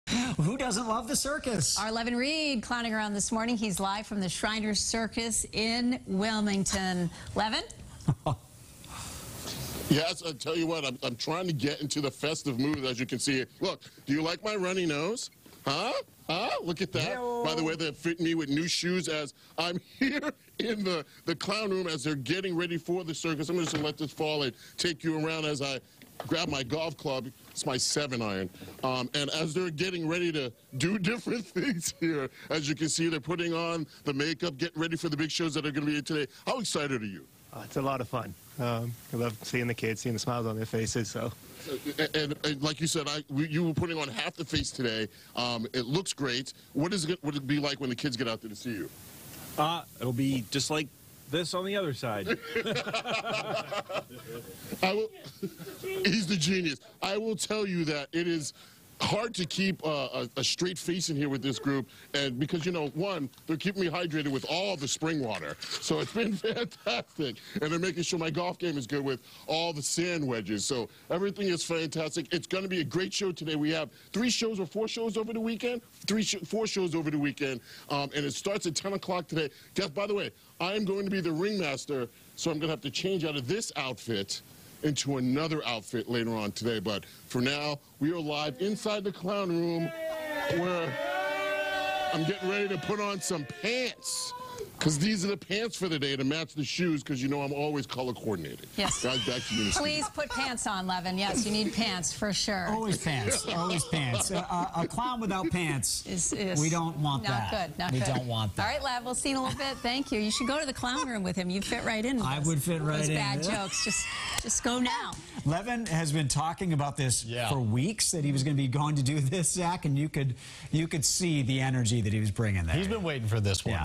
Levan Reid clowns around with Shriners Circus
WBZ-TV's Levan Reid goes behind the scenes with the Shriners Circus in Wilmington